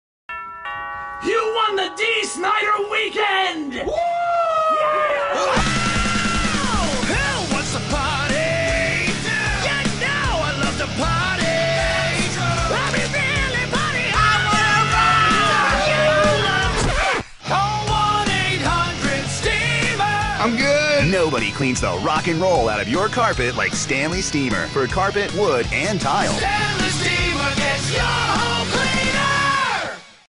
Stanley Steemer Rock n Roll Carpet 30
Jonathan D Gaietto in a Stanley Steamer Commercial